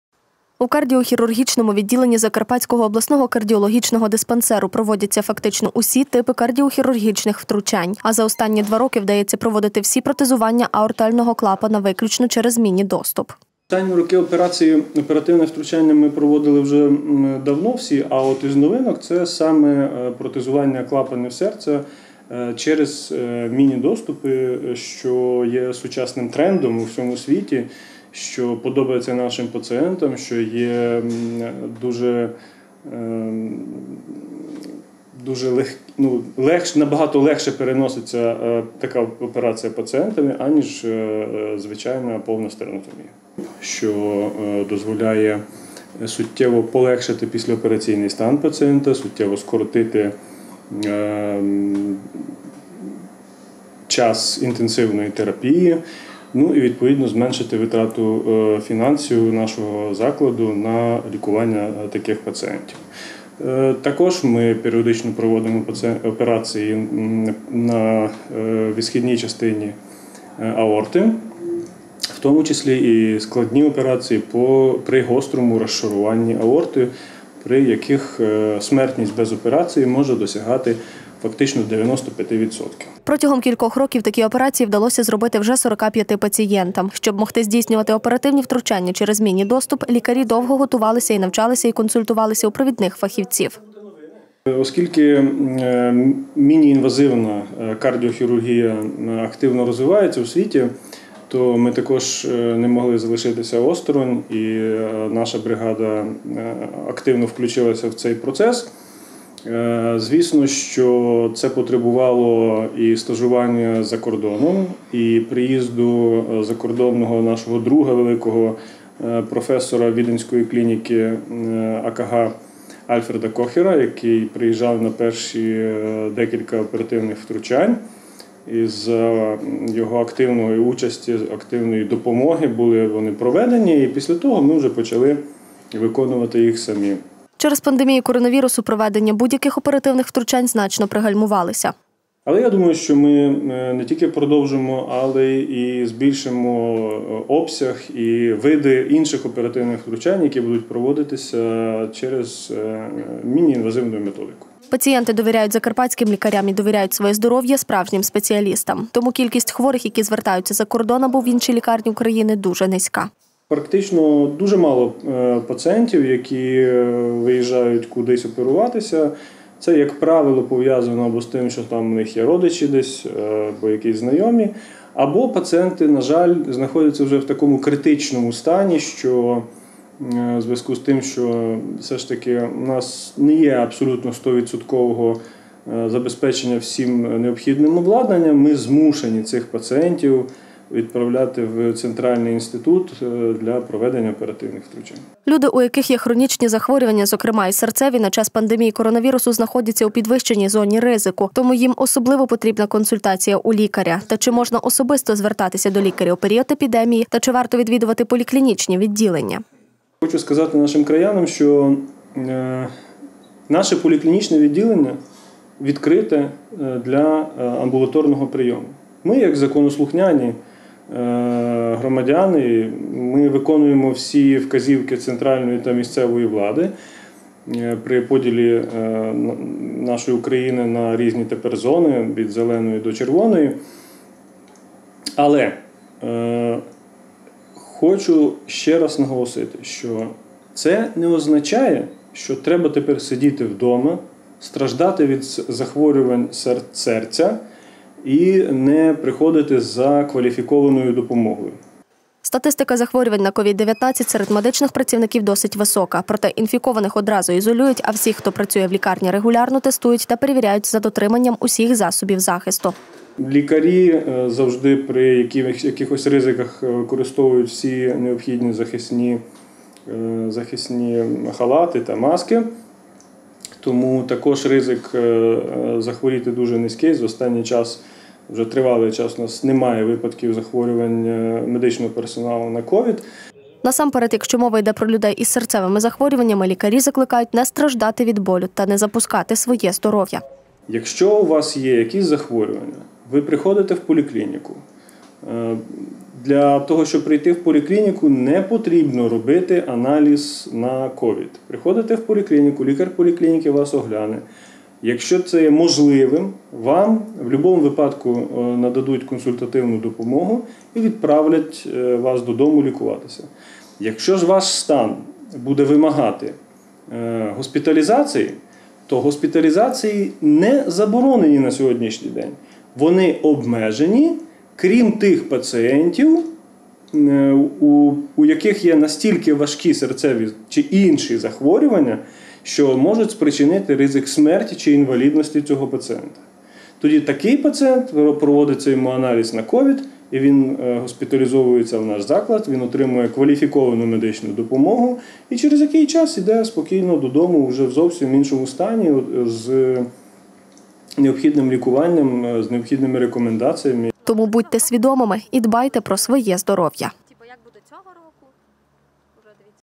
Бережіть здоров’я. Ендопротизування аорти, збереження здоров’я та поради лікарів.
Минулого року у Закарпатському обласному кардіологічному диспансері провели операцію у новому напрямку кардіохірургії — з ендопротезування аорти. У цьому закладі намагаються іти в ногу з часом, і лікувати своїх пацієнтів найсучаснішими методами. Важливо наголосити,  що у період пандемії варто особливо дбати про своє здоров’я, тим паче людям, які мають хронічні захворювання, адже вони знаходяться у підвищеній зоні ризику.